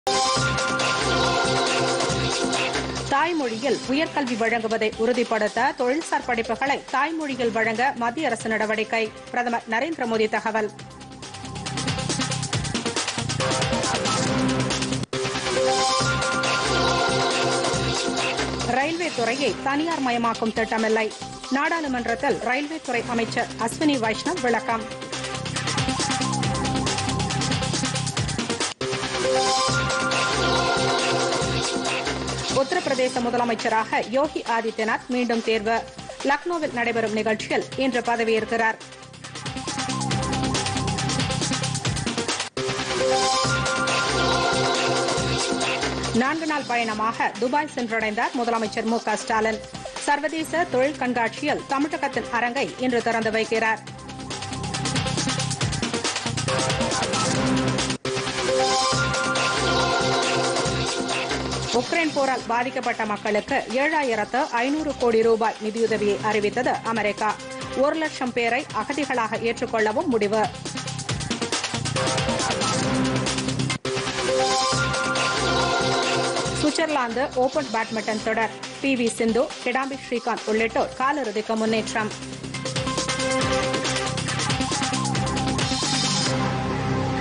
பொதிகை காலை 8.00 மணி தலைப்புச் செய்திகள் [25.03.2022]PodhigaiTamilNews
1)  தாய்மொழியில் உயர்கல்வி வழங்குவதை உறுதிப்படுத்த தொழில்சார் படிப்புகளை தாய்மொழியில் வழங்க  மத்திய அரசு நடவடிக்கை - பிரதமர்  நரேந்திரமோதி  தகவல்

2) ரயில்வேத்துறையை தனியார்மயமாக்கும் திட்டமில்லை - நாடாளுமன்றத்தில் ரயில்வேத்துறை அமைச்சர் அஸ்வினி வைஷ்ணவ் விளக்கம்

3) உத்தரப்பிரதேச முதலமைச்சராக யோகி ஆதித்யநாத் மீண்டும்  தேர்வு - லக்னோவில் நடைபெறும் நிகழ்ச்சியில் இன்று பதவியேற்கிறார்

4) நான்குநாள் பயணமாக துபாய் சென்றடைந்தார் முதலமைச்சர் மு க ஸ்டாலின் - சர்வதேச தொழில் கண்காட்சியில் தமிழகத்தின் அரங்கை இன்று திறந்துவைக்கிறார்

5) உக்ரைன் போரால் பாதிக்கப்பட்ட மக்களுக்கு ஏழாயிரத்து 500 கோடி ரூபாய் நிதியுதவியை அறிவித்தது  அமெரிக்கா - ஒரு லட்சம் ‍பேரை அகதிகளாக ஏற்றுக்கொள்ளவும் முடிவு

6) சுவிட்சர்லாந்து ஓபன் பேட்மிண்டன் தொடர் - பி வி சிந்து, கிடாம்பி ஸ்ரீகாந்த் உள்ளிட்டோர் காலிறுதிக்கு முன்னேற்றம்